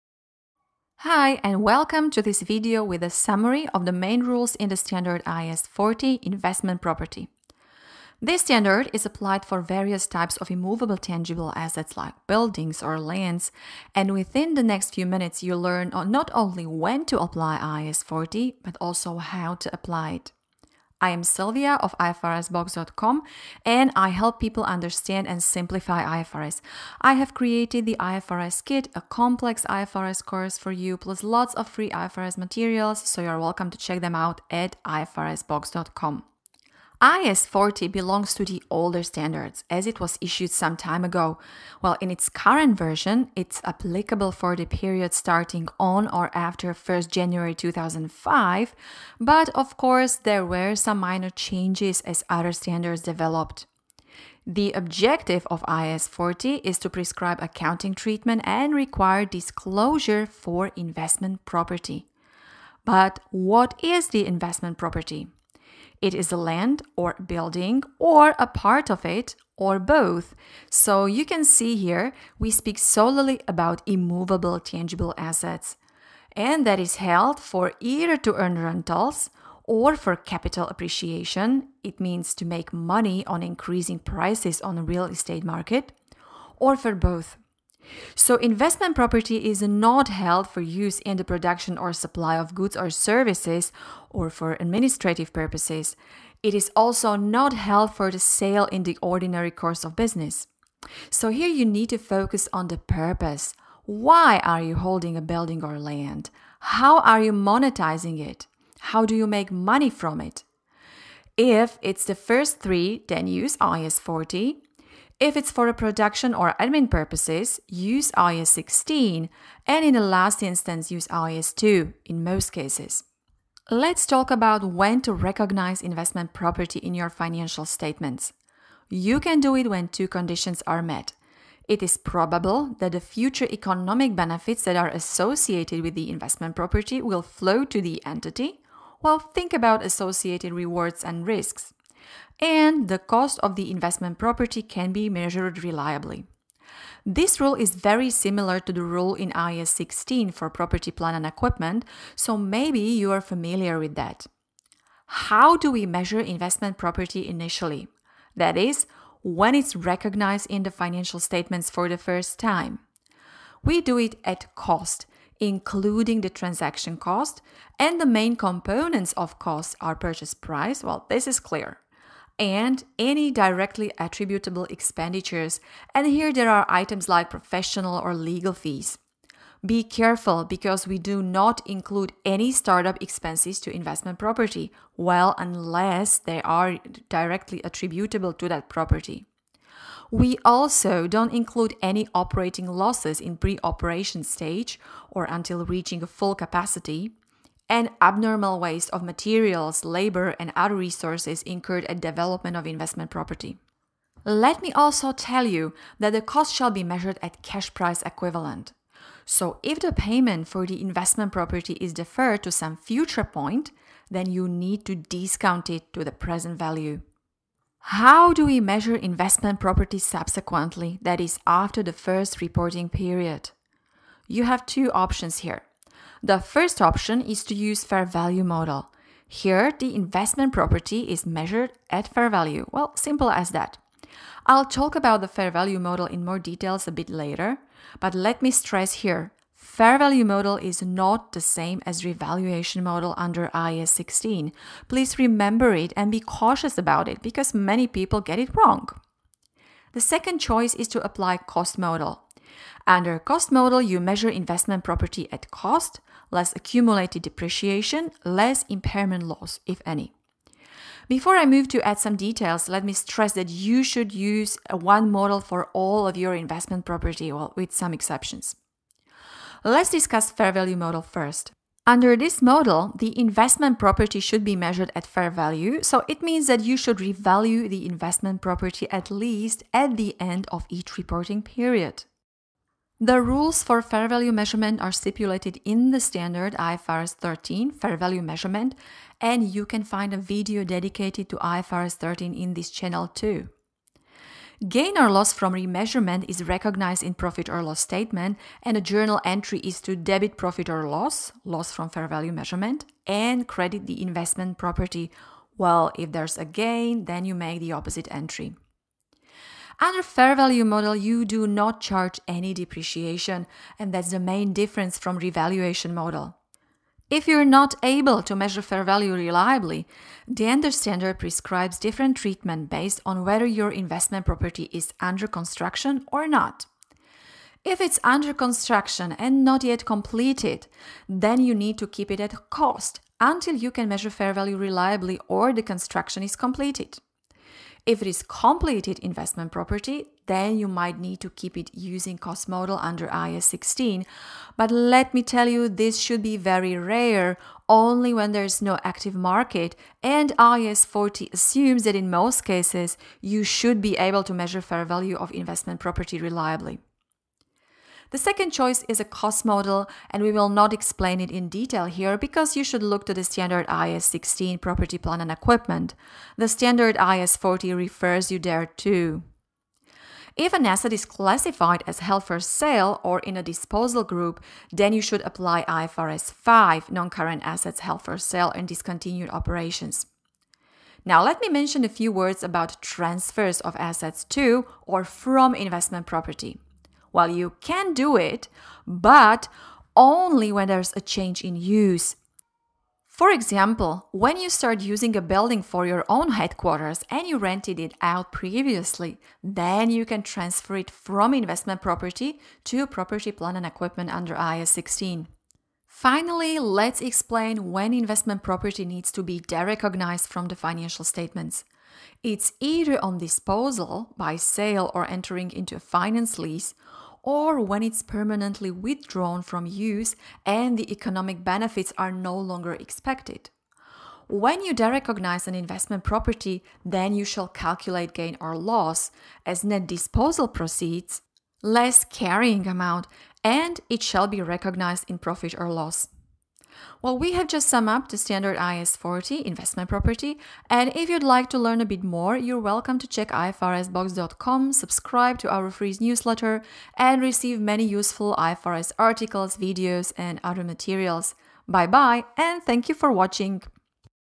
Summary of IAS 40 Investment Property - applies in 2024
This is just the short executive summary of IAS 40 and does NOT replace the full standard - you can see the full text on IFRS Foundation's website.